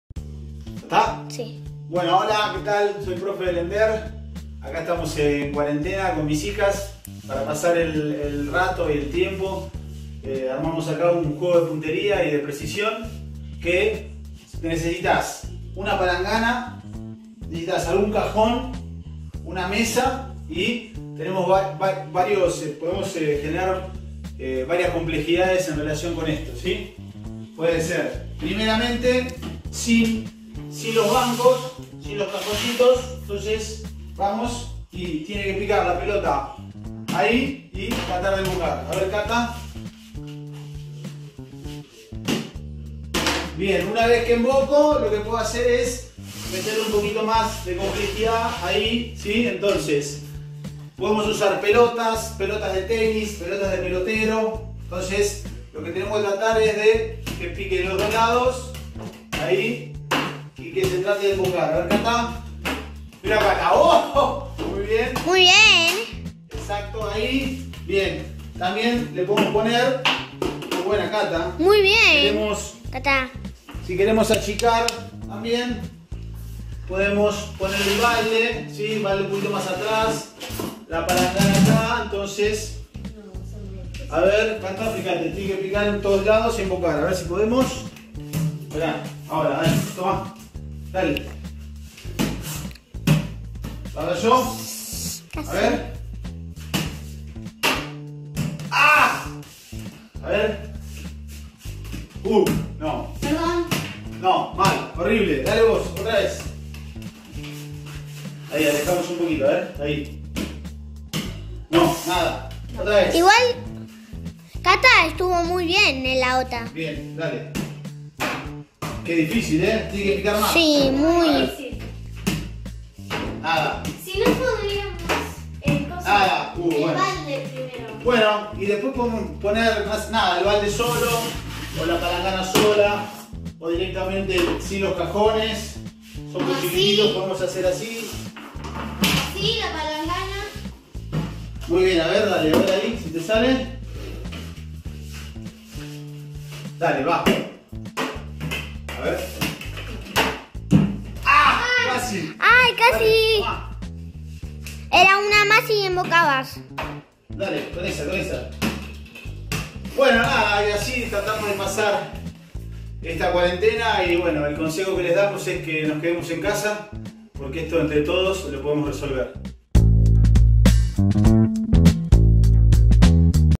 MeQuedoEnCasa jugando
El profesor Pablo Mascareña nos deja juegos para realizar con los pequeños y pequeñas en casa.